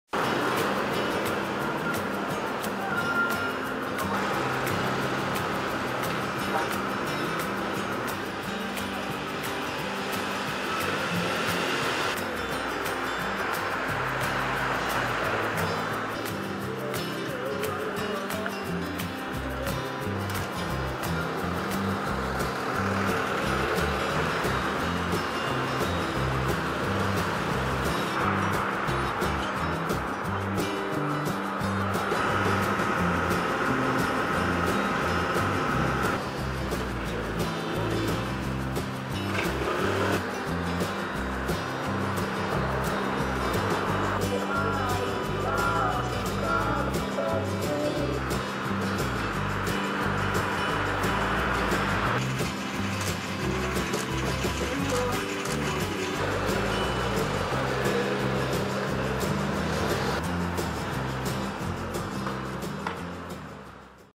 가평군 중심 시가지 2차 경관(간판) 정비사업 준공
가평군은 가평읍 가화로 일대 294m 구간에 도시미관을 저해하는 불법 및 노후 간판들을 철거하고 벽면이용간판 98개, 돌출간판 156개를 신규 설치했습니다.